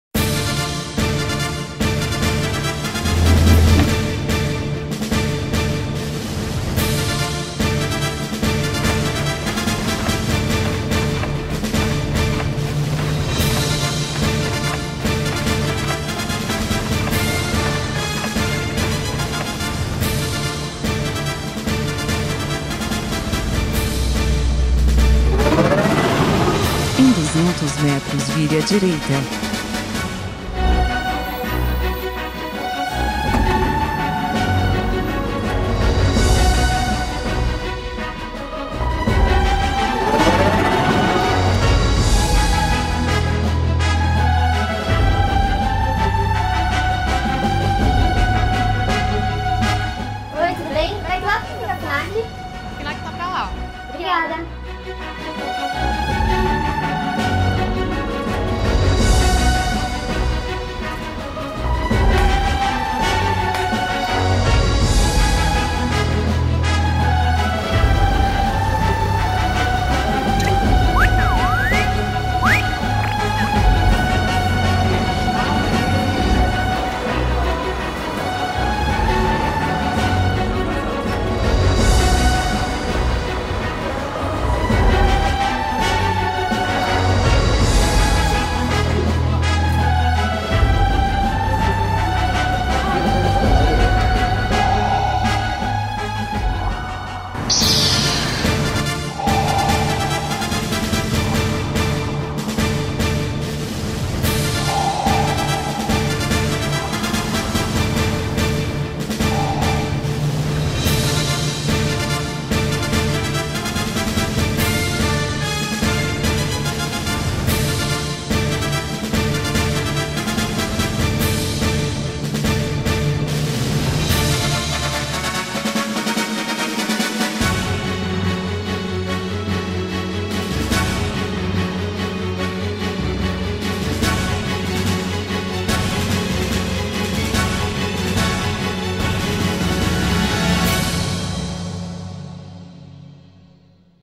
Star Wars: a galáxia se encontra na Fnac
Jedi, Stormtroopers e outros personagens do universo Star Wars invadiram a Fnac Pinheiros, em São Paulo!

Venha você também para o nosso lado da Força ;)